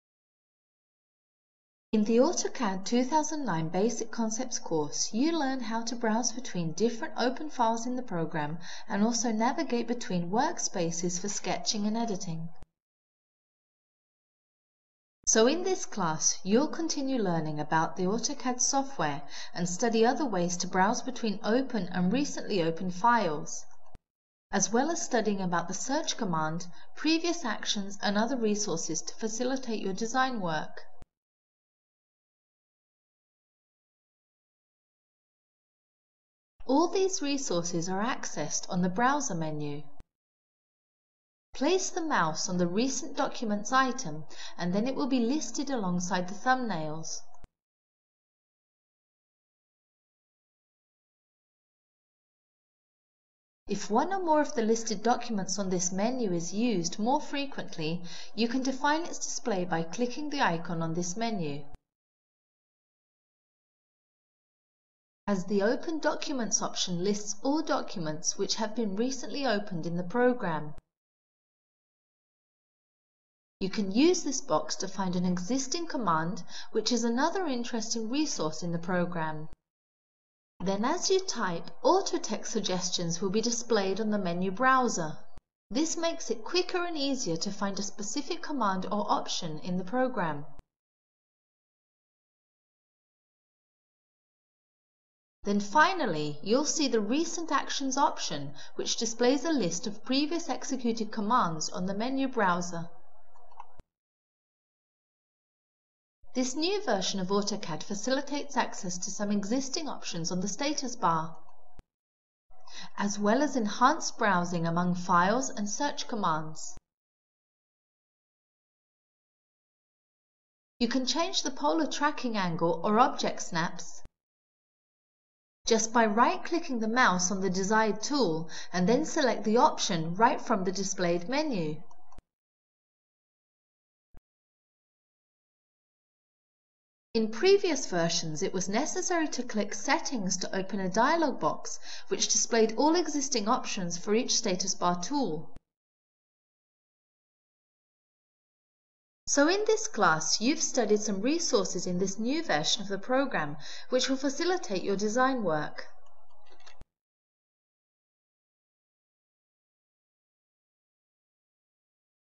Advanced Interface Options - M1A1
In this class, you will learn about some existing resources in the program interface to facilitate browsing among documents, access commonly used commands, and some more program settings.